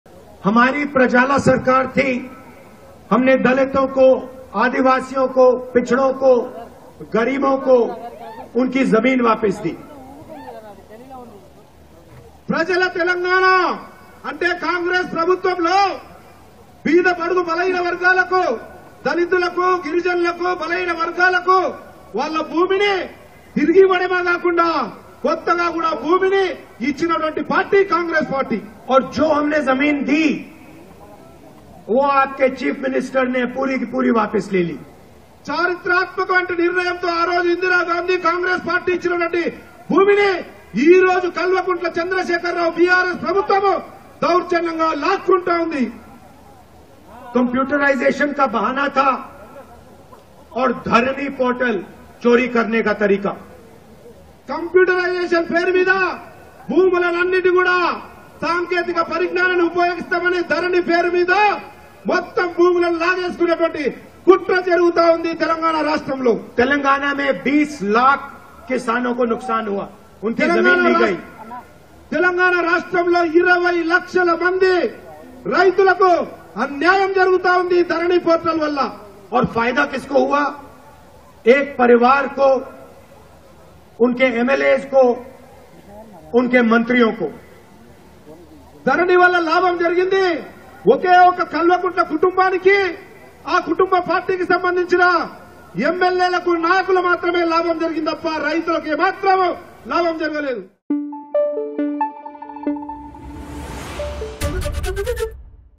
Rahul Gandhi About Dharani Portal | ధరణి పోర్టల్ పై రాహుల్ గాంధీ సంచలన కామెంట్స్ | ABP Desam
ధరణి పోర్టల్ ద్వారా పేదల భూములు కేసీఆర్ లాక్కుంటున్నారని రాహుల్ గాంధీ విమర్శించారు. తాము అధికారంలోకి వస్తే ధరణి పోర్టల్ రద్దు చేస్తామని హామీ ఇచ్చారు.

Rahul Gandhi About Dharani Portel | ధరణి పోర్టల్ పై రాహుల్ గాంధీ సంచలన కామెంట్స్ | ABP Desam